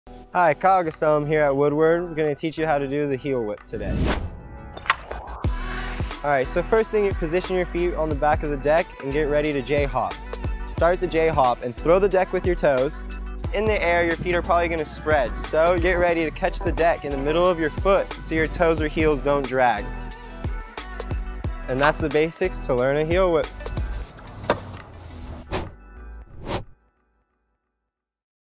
Mongoose Trick Tips: Heelwhip
tricktiptuesday with Kyle! In this video, Kyle walks us through how to heelwhip on a freestyle scooter. The trick tip series was recorded at Camp Woodward East with park riders.